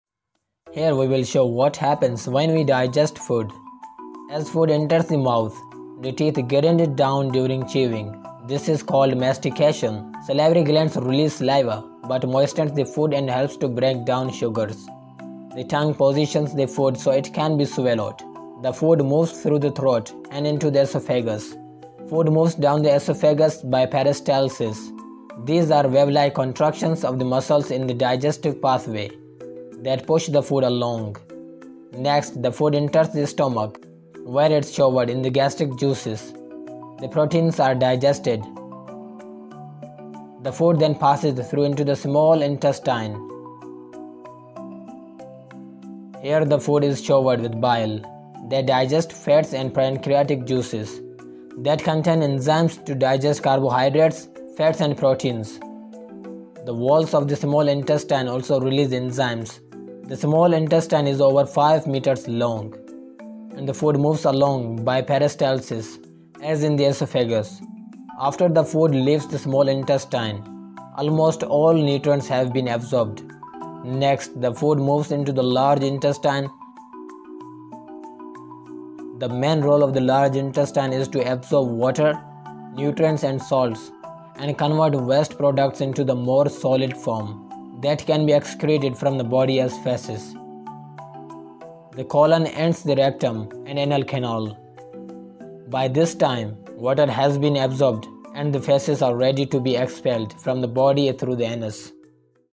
Human digestive system - How does our body digest food ?
Digestive system...

Here we'll show what happens when we digest food. As food enters the mouth, the teeth grind it down during chewing. This is called mastication. Salivary glands release saliva but moistens the food and helps to break down sugars.
 The tongue positions the food so it can be swallowed. The food moves through the throat and into the esophagus. Food moves down the esophagus by peristalsis. These are wavelike contractions of the muscles in the digestive pathway that push the food along.
 Next, the food enters the stomach where it's showered in gastric. Juices and proteins are digested. The food then passes through into the small intestine. Here the food is showered with bile that digests fats and pancreatic juices.
 That contain enzymes to digest carbohydrates, fats and proteins. The walls of the small intestine also release enzymes. The small intestine is over 5 meters long and the food moves along by peristalsis, as in the esophagus.
After the food, leaves the small intestine. Almost all the nutrients have been absorbed. Next, the food moves into the large intestine. The main role of the large intestine is to absorb water, nutrients and salts and convert waste products into a more solid form that can be excreted from the body as feces.
 The colon ends in the rectum and anal canal. By this time, water has been absorbed and the feces are ready to be expelled from the body through the anus.